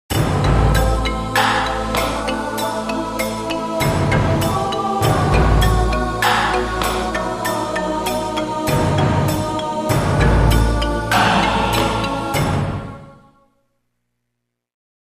アマダクション若園ＤＶＤオープニング
山岸堂の前身「アマダクション若園」のＤＶＤで流れたＣＭ映像。